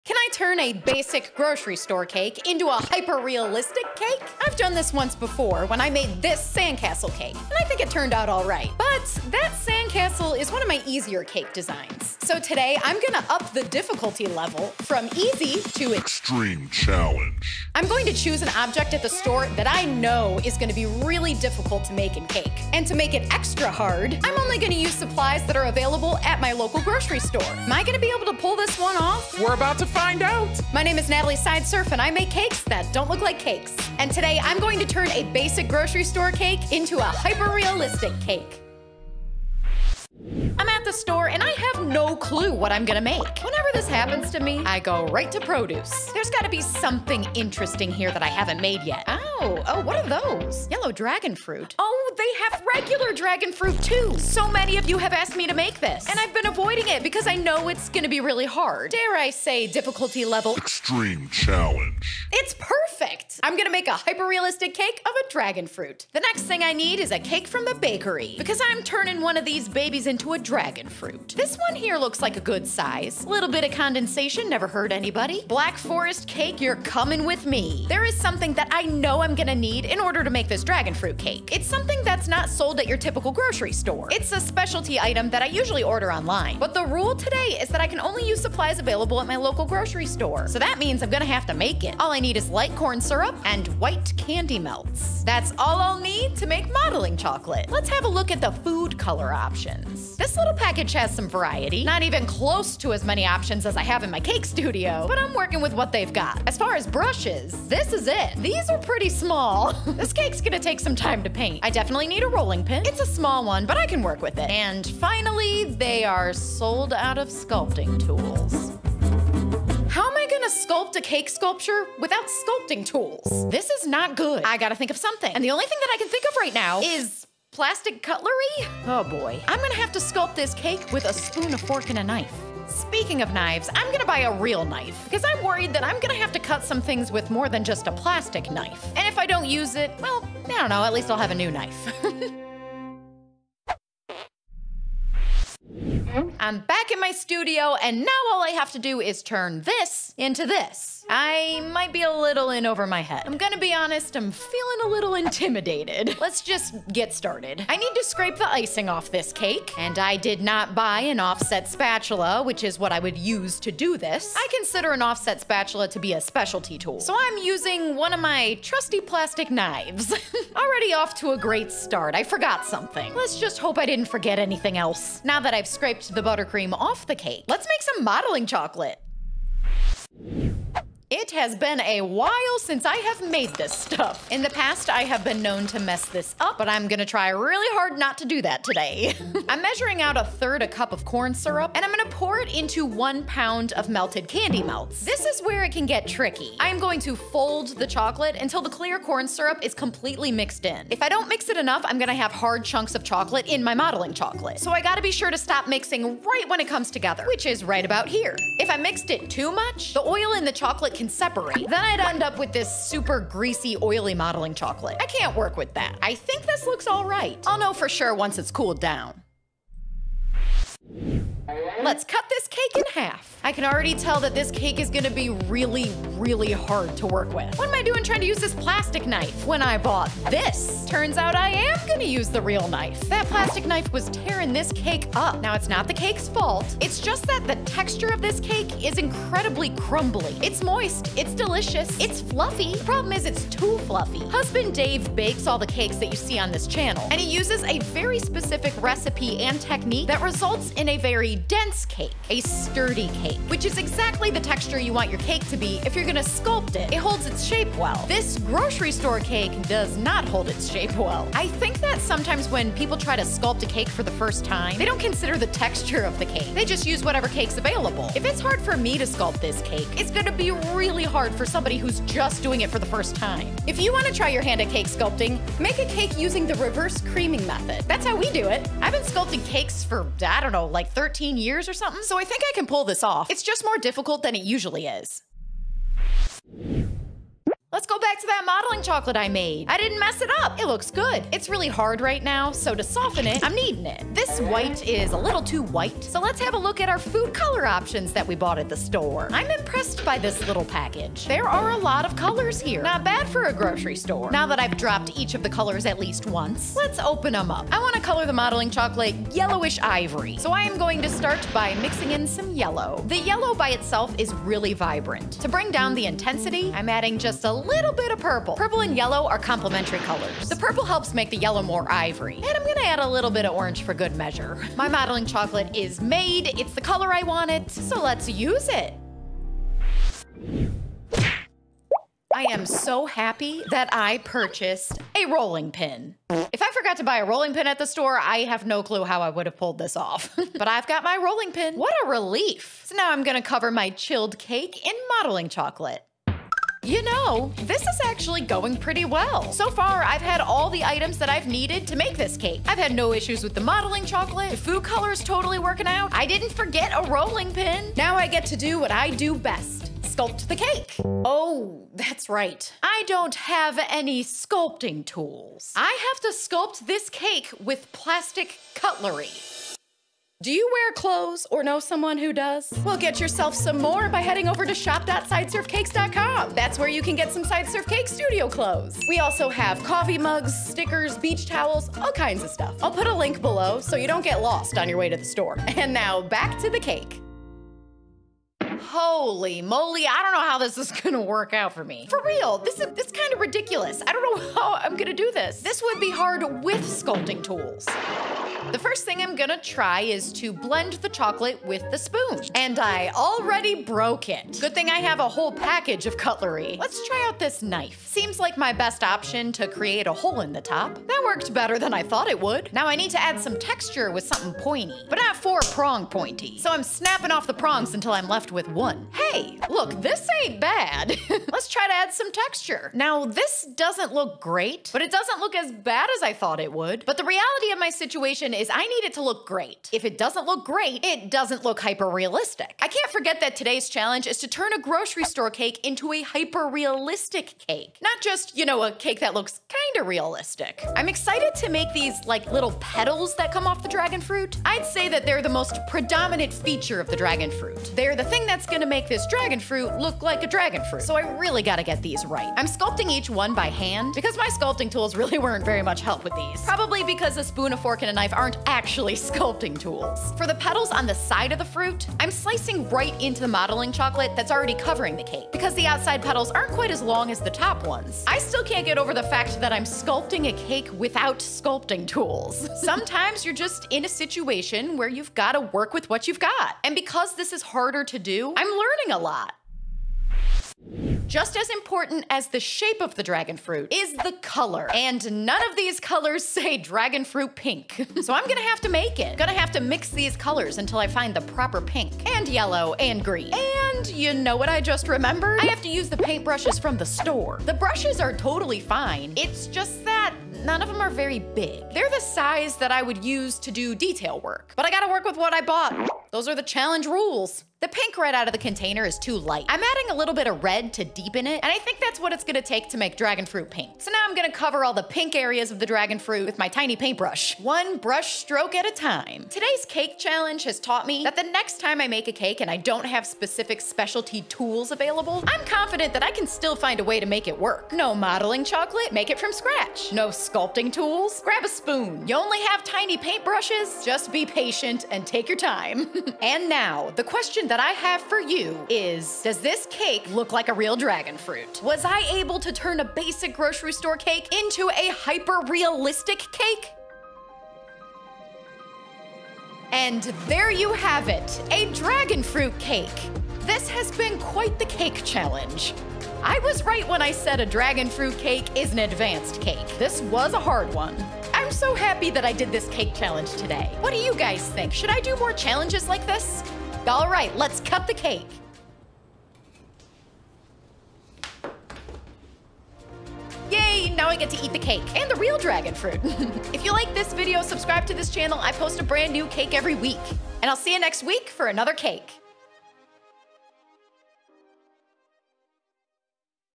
Can I Turn a Grocery Store Cake Into a Hyperrealistic Cake?
This week, I'm challenging myself to turn a regular grocery store cake into a hyperrealistic cake using ONLY items I found at my local grocery store!